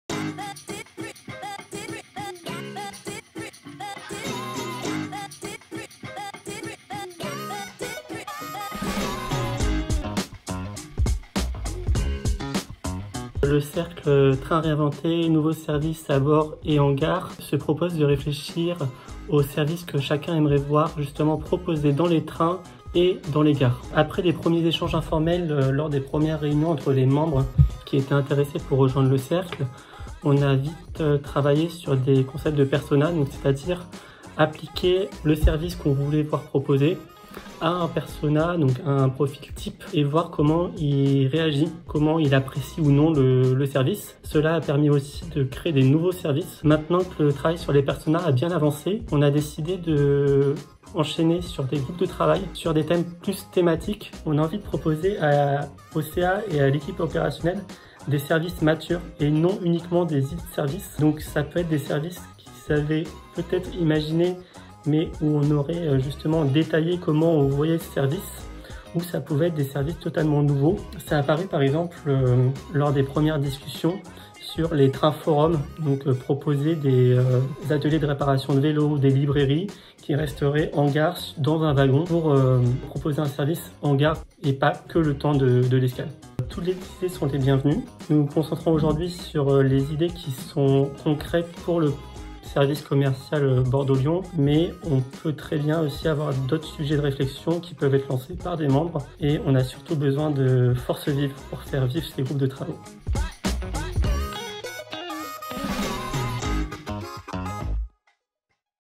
Cercles de Sociétaires - cercle Train réinventé et nouveaux services à bord et en gare
Cercles de Sociétaires

Être sociétaire chez Railcoop, c’est aussi avoir la possibilité de s’investir dans des cercles de réflexion et de participer directement au projet de la coopérative.🚆

Aujourd’hui, découvrez le cercle « Train réinventé, nouveaux services à bord et en gare » !
Ce cercle s’intéresse aux services et à l’accueil à bord et en gare que la coopérative pourra proposer.
Les membres de ce cercle imaginent l’expérience des futurs voyageurs Railcoop.

Vous êtes sociétaire ? Vous souhaitez proposer vos idées ou vous engager dans ce cercle ? Ses membres seront ravis de vous accueillir !

Participez au développement du train au bénéfice de tous 🤝 en souscrivant sur railcoop.fr